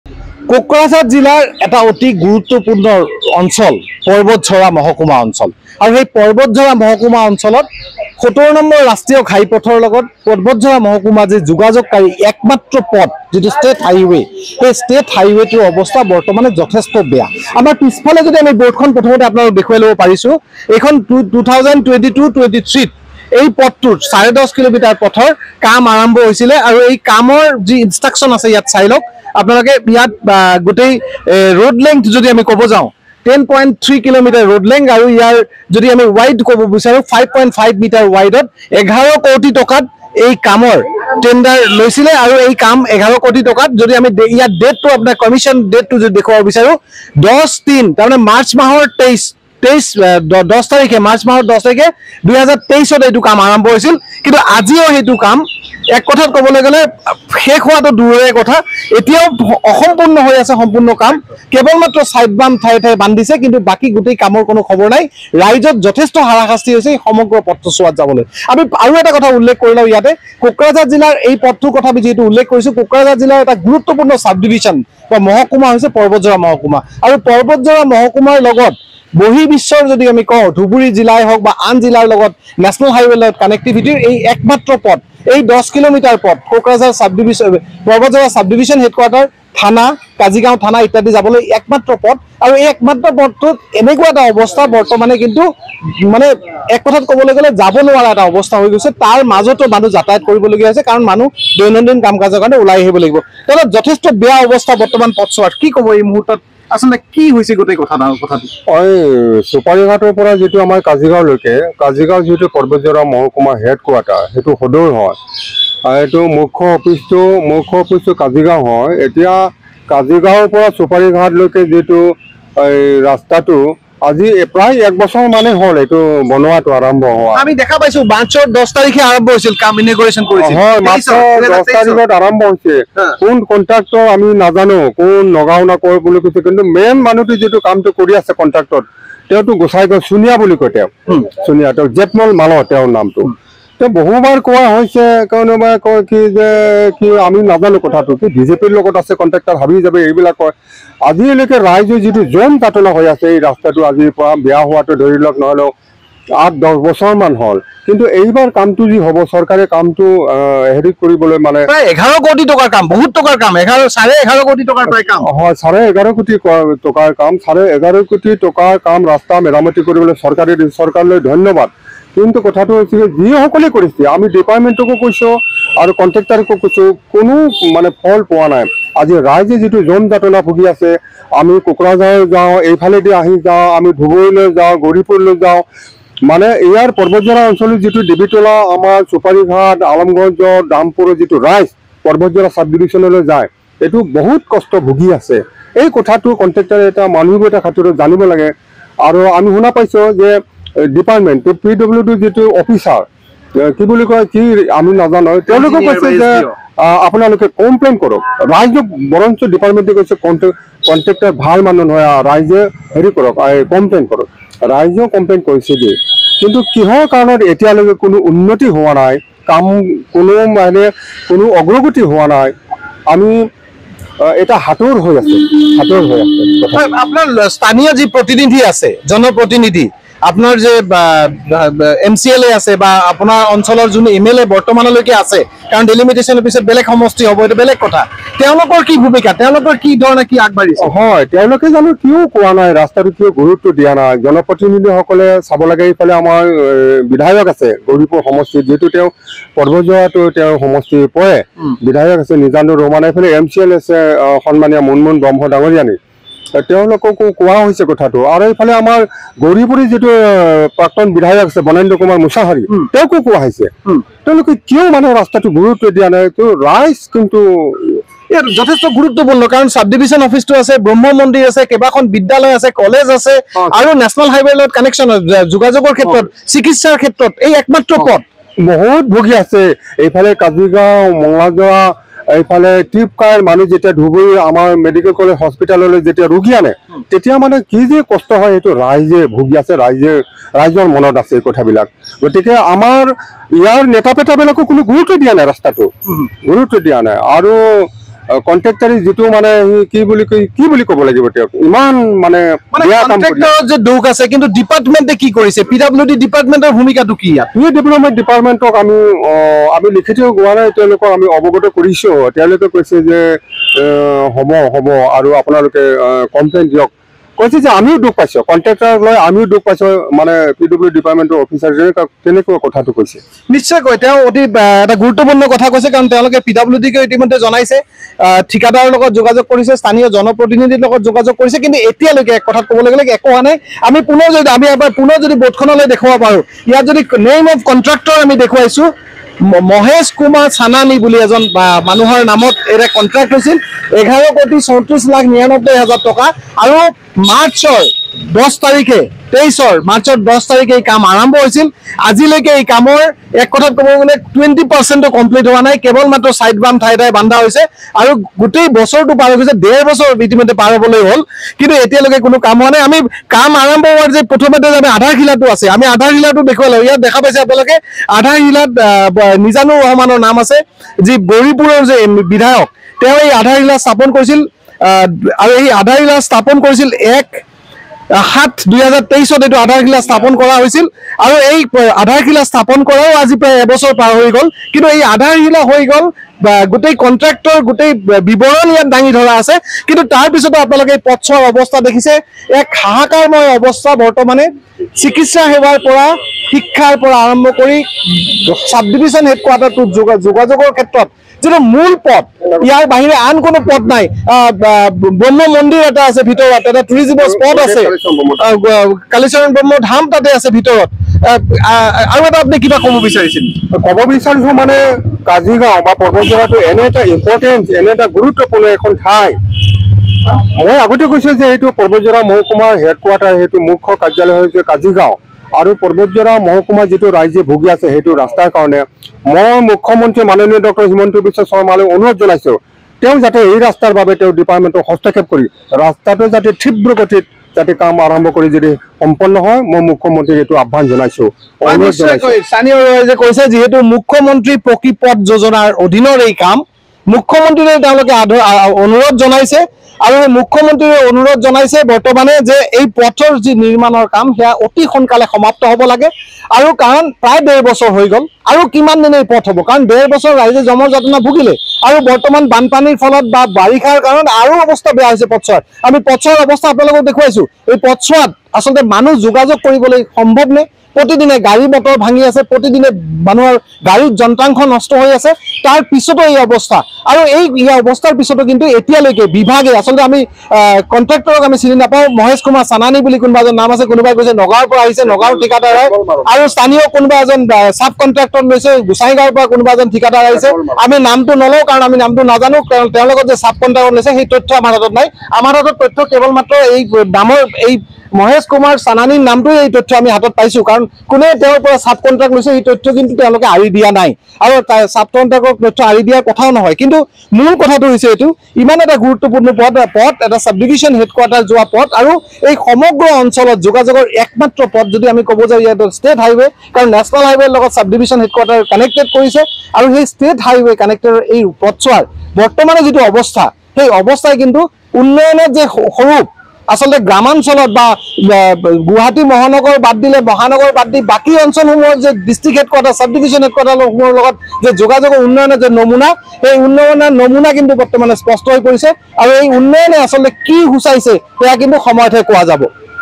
Kokrajhar News : পথ নিৰ্মাণত ব্যাপক বিসংগতি | N18V
Kokrajhar News | পথ নিৰ্মাণত ব্যাপক বিসংগতি। কোকৰাঝাৰ জিলাৰ পৰ্ব্বতঝোৰা মহকুমা সদৰ সংযোগী পথৰ দুৰৱস্থা।